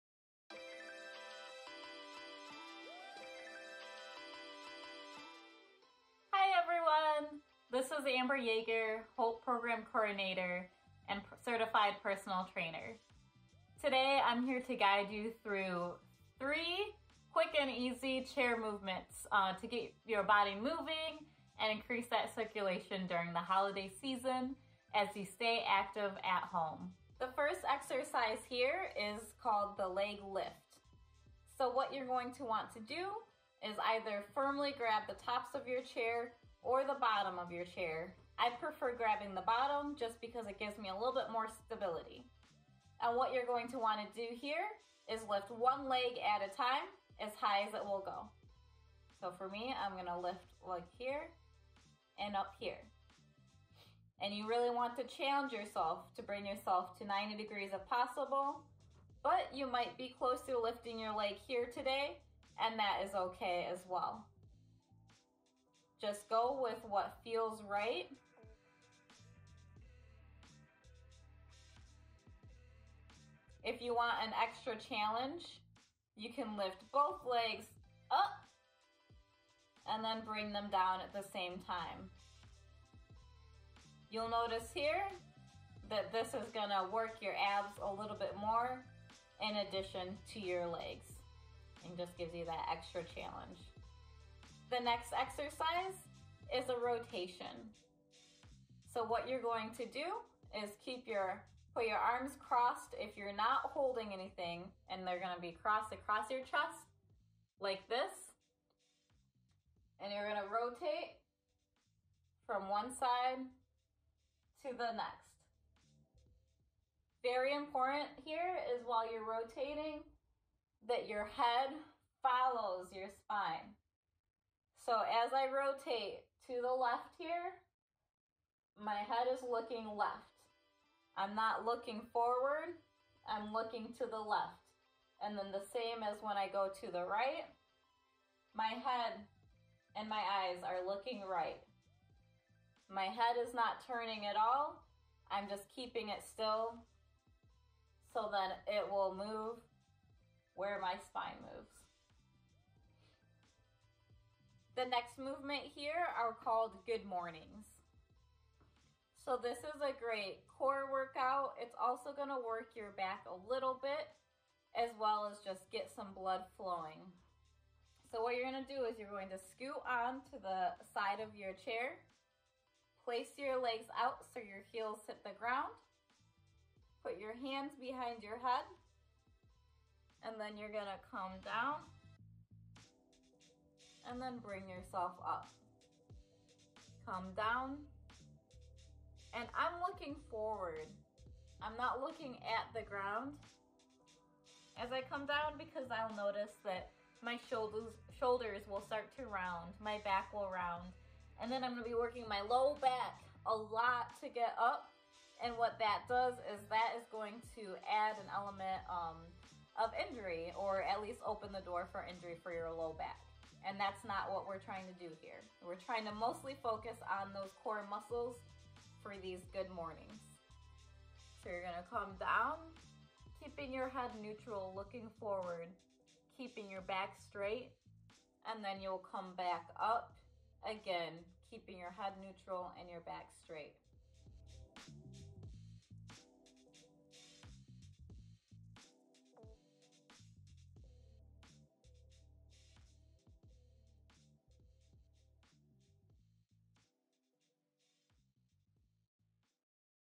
HOPE Home and Healthy for the Holidays Part 2
In part two of Home and Healthy for the Holidays we look at three chair exercises that you can incorporate into your at home workout routine.